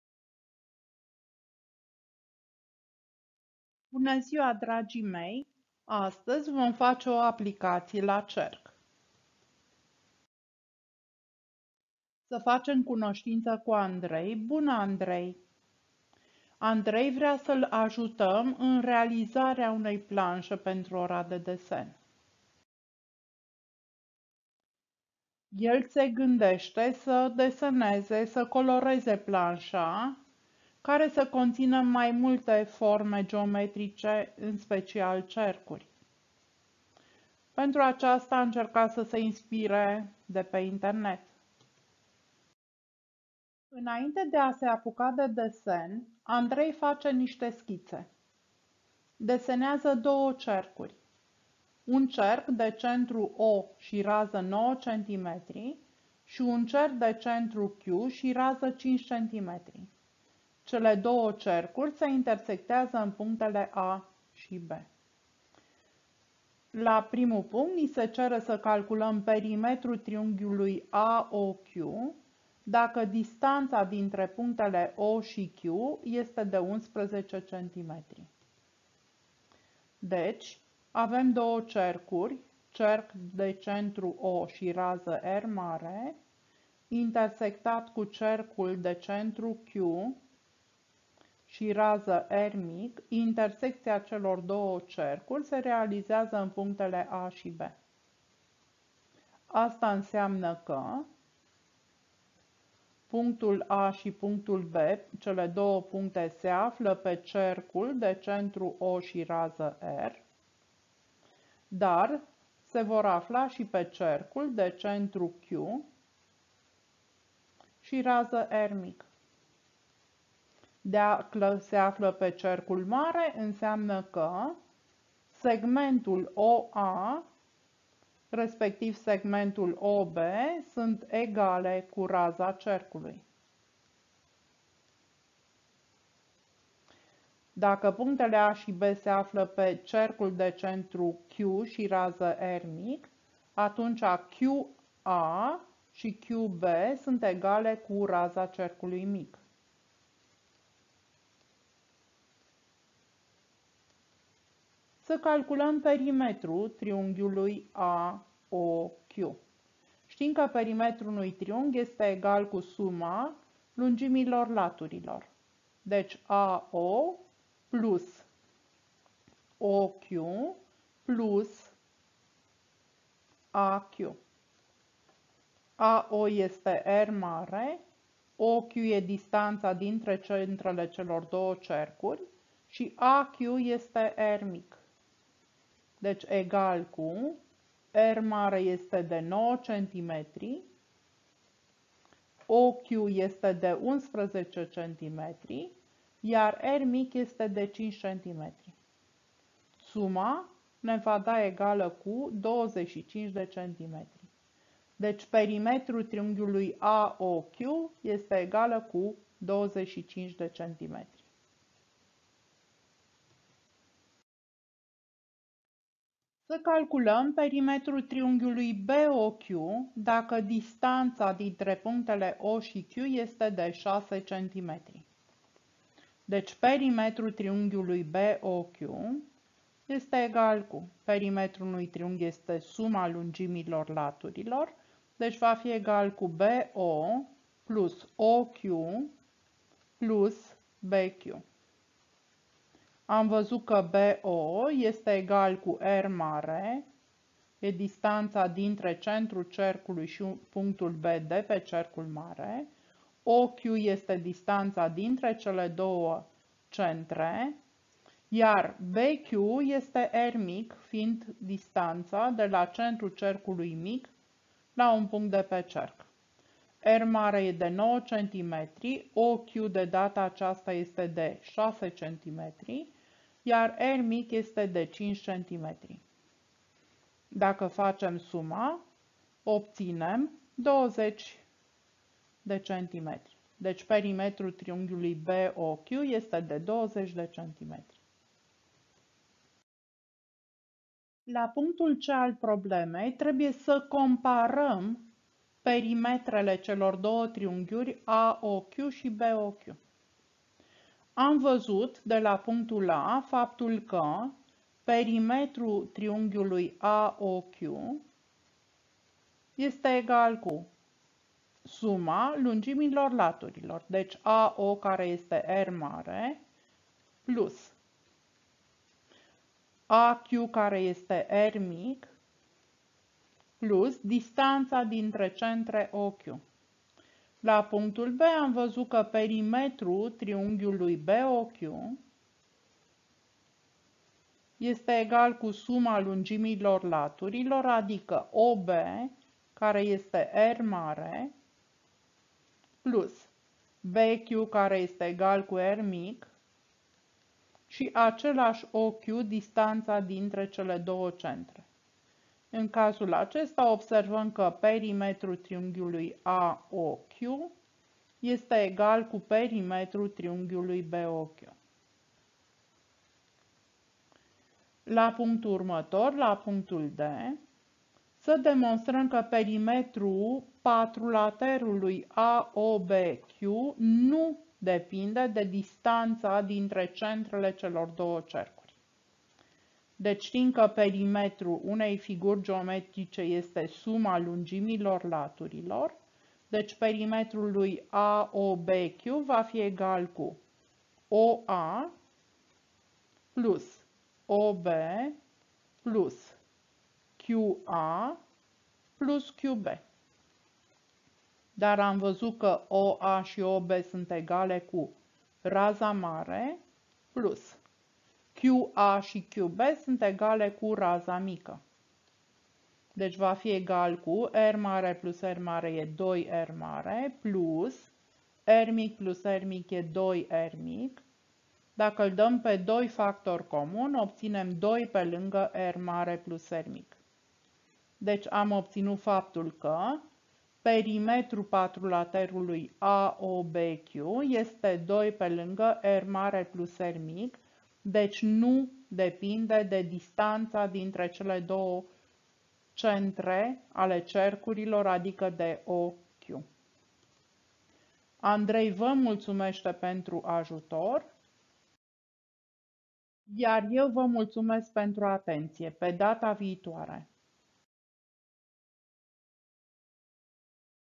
Cerc - Aplicație
Resursa propusă a fost concepută atât ca suport teoretic pentru predarea la clasă, cât și pentru studiul individual.
Activitatea poate fi organizată la clasă frontal sau pe grupe, dar materialul poate fi folosit foarte bine și pentru predarea online sau hibrid, fiind un bun suport pentru realizarea unei secvențe de lecție de 8-10 minute.
Materialul propus poate fi utilizat singur sau ca parte componentă a unei activități mai ample de învățare, activitate ce presupune atingerea mai multor competențe.
Activitatea de învățare vizată de acest material constă în determinarea unor lungimi de segmente utilizând informații cuprinse în reprezentările geometrice.

Autor: Rodica Doinita Cocalea